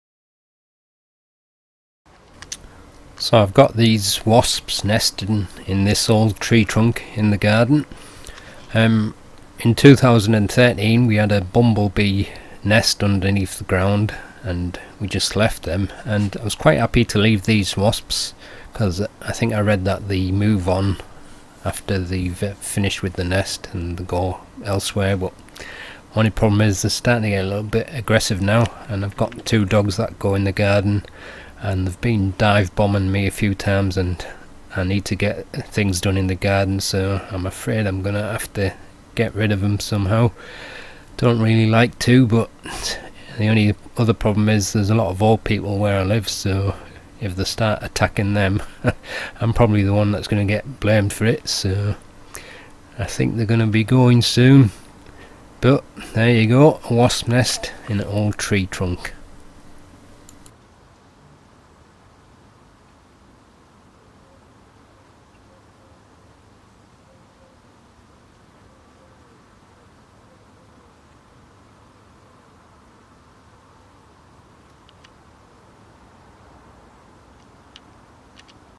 Wasp Nest In Old Tree Trunk In Garden
This is a Wasp nest that is in an old tree trunk in my garden.  In 2013 there was a Bumble Bee nest underground, here is the video I made of them

I was happy enough to leave the wasps as I think they just move on once they have finished with their nest but they seem to be getting a little more aggressive now and dive bomb me when I go in the garden, thankfully they have not stung me yet.  I have two dogs that go in the garden too so getting a little worried.  I have spoken to a pest control guy who said to dust the nest with wasp powder, I am not too happy about having to kill them but as there is also a lot of elderly people in gardens nearby he said if they get stung and I am known to have had them nesting in the garden I could get blamed for it.   I know lots of people dont like wasps but they can also do some good by keeping other pest insects down and to a lesser extent still pollinated plants, if there was a way to move them without killing them then that would be better I guess.